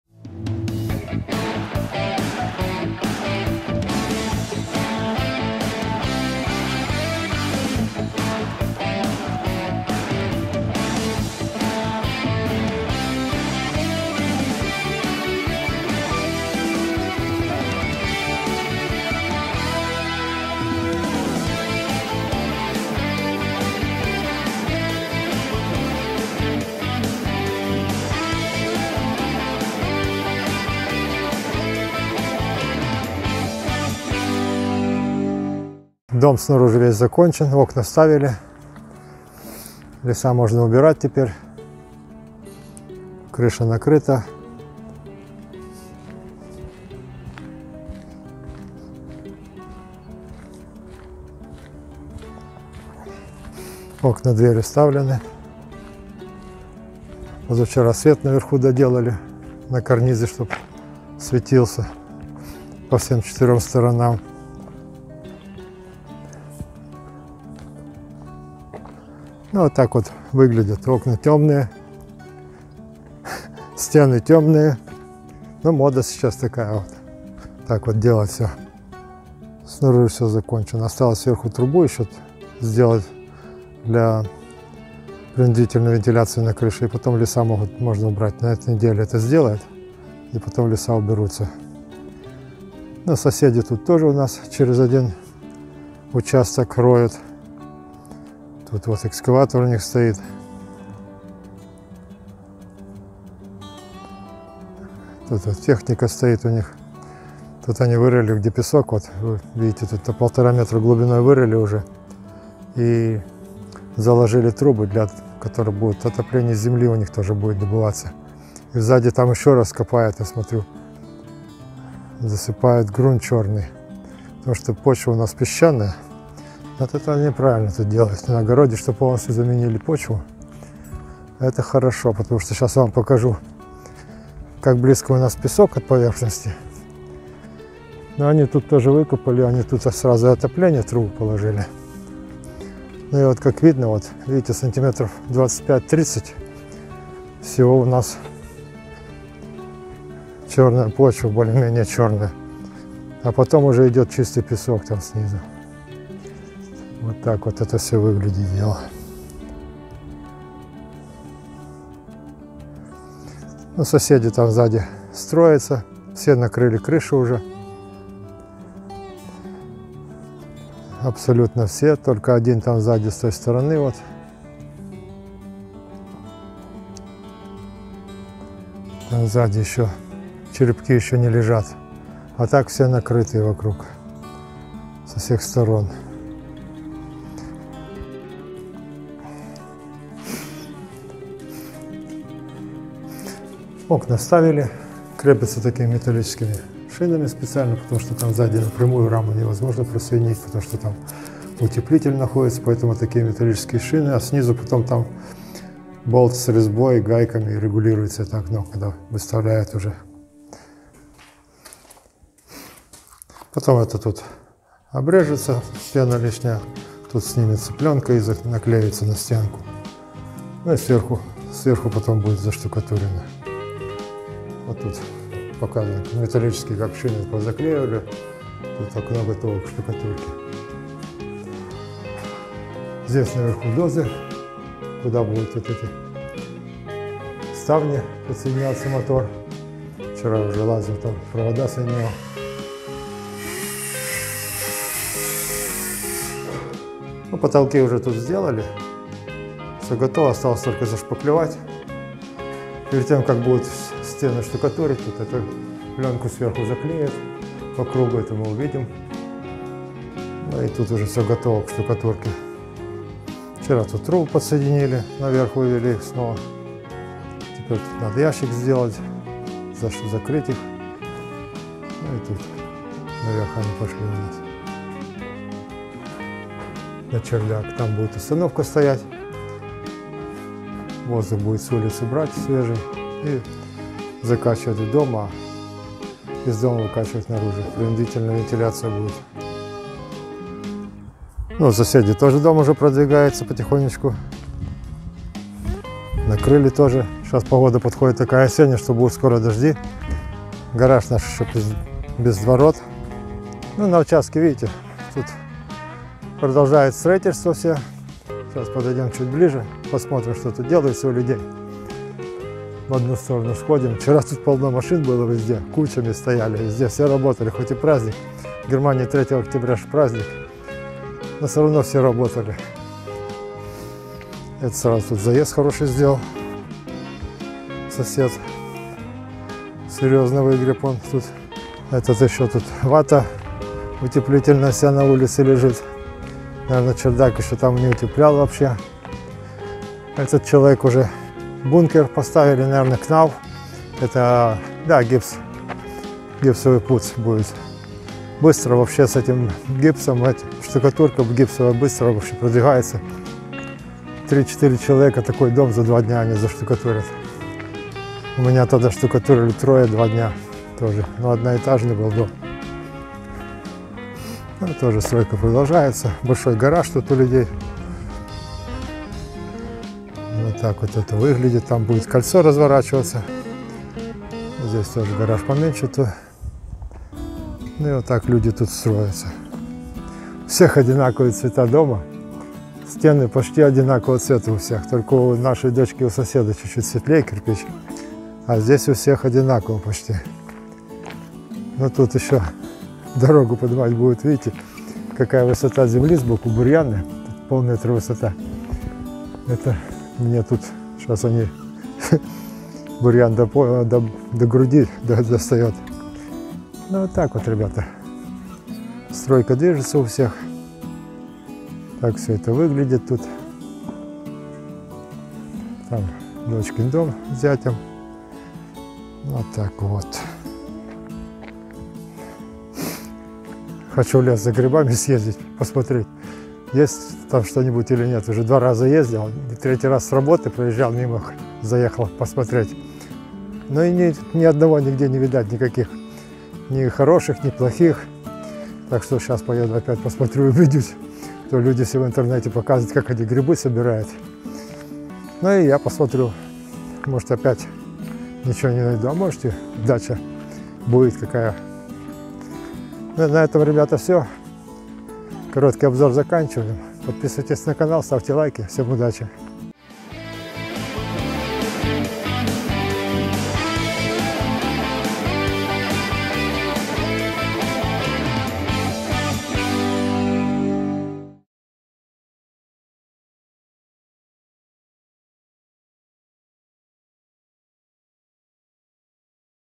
Крышу накрыли, окна вставили Часть 14
Германия. Продолжаю обзор строительства дома. Дочь с зятем строят для себя дом формы Городская Вилла. В этом ролике увидите как накрыли крышу и вставили окна. Потолок утеплили и подшили. Что делают соседи ну и всего понемногу.